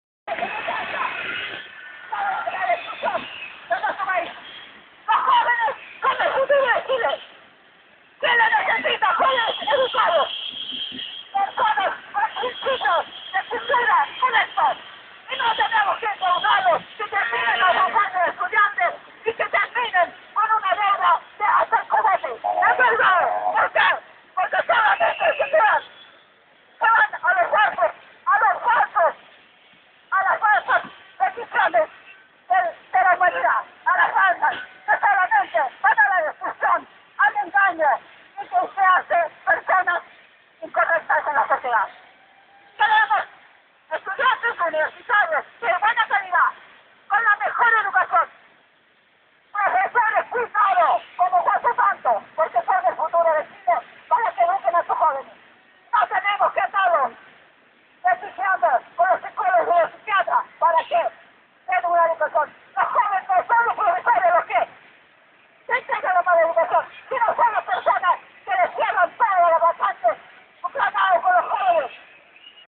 abuela encarando a la policia
abuela encarando a la policía en su cara, en la manifestación de estudiantes y profesores en stgo de Chile, 30 de junio 2011